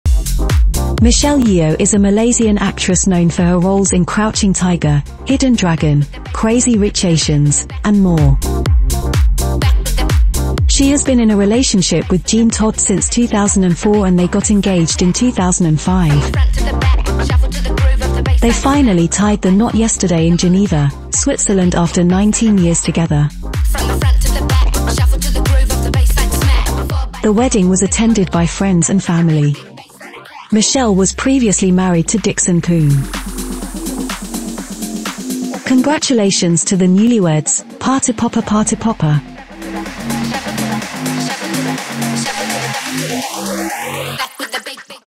Michelle Yeoh and Jean Todt are MARRIED
Michelle Yeoh is a Malaysian actress known for her roles in Crouching Tiger, Hidden Dragon, Crazy Rich Asians, and more.

She has been in a relationship with Jean Todt since 2004 and they got engaged in 2005. They finally tied the knot yesterday in Geneva, Switzerland after 19 years together.

The wedding was attended by friends and family.

Michelle was previously married to Dickson Poon.

Congratulations to the newlyweds!🎉🎉